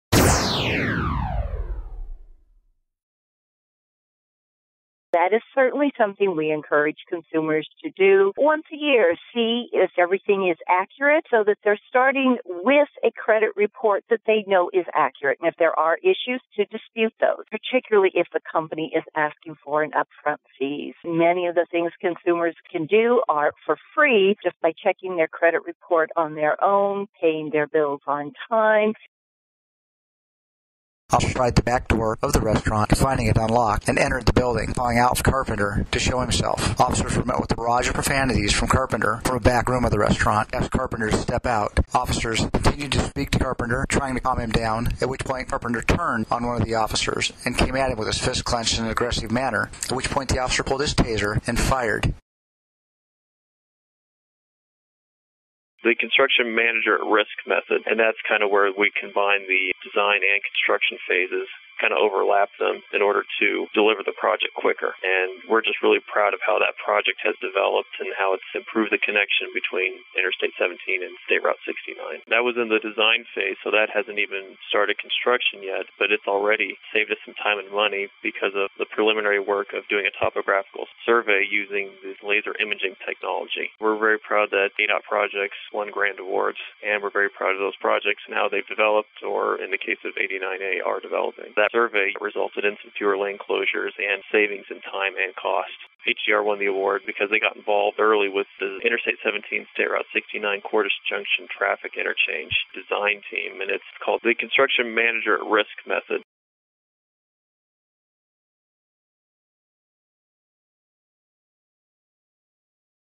Bill's Newscast: ADOT Wins Engineering Excellence Awards
$50 Million Project one of ADOT's major milestones 0f 2013.